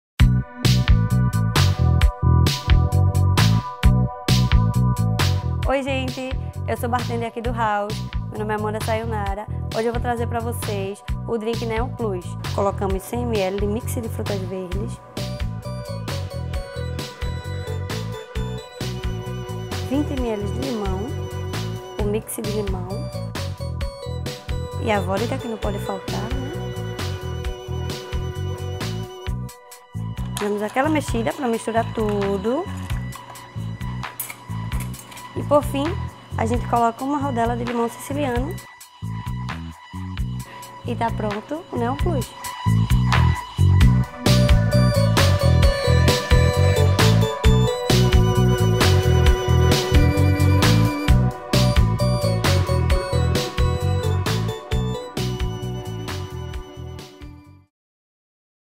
Drink da Sexta: Neon Plus
Assista ao programa DiarioDePernambucoTV todo sábado às 10h

Aprenda a fazer o drink Neon Plus do Haus.